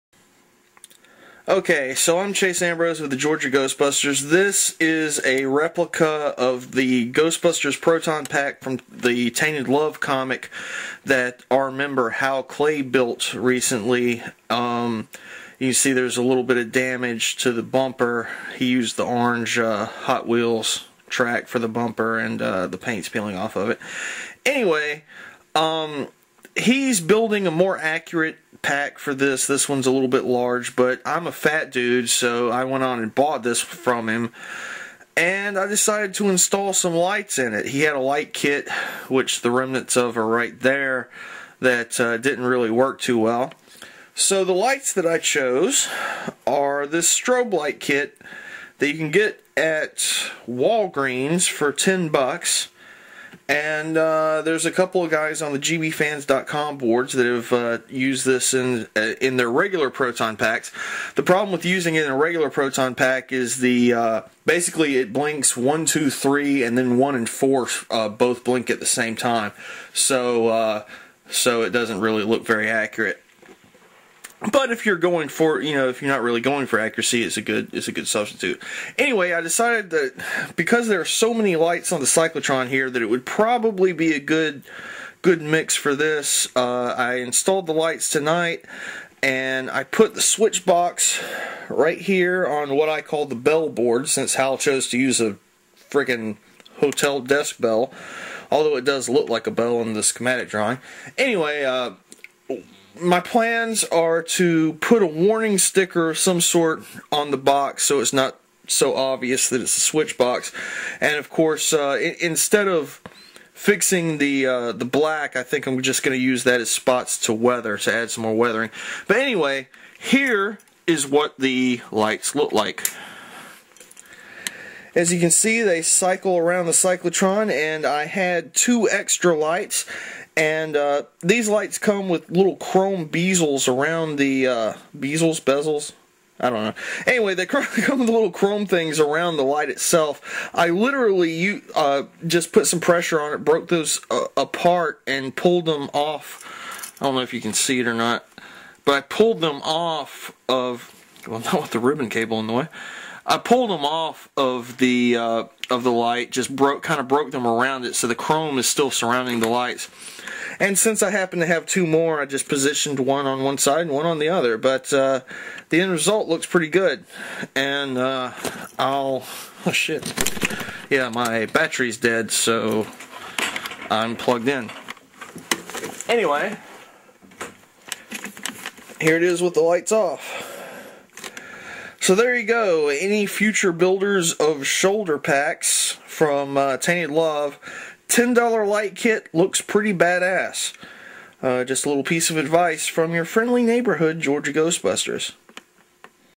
pack.wmv
This is a video of a 10 dollar light kit in a shoulder mounted Proton Pack based on the IDW Ghostbusters comic titled"Tainted Love" The kit is a Halloween seasonal item available at Walgreens...at least this year (2010)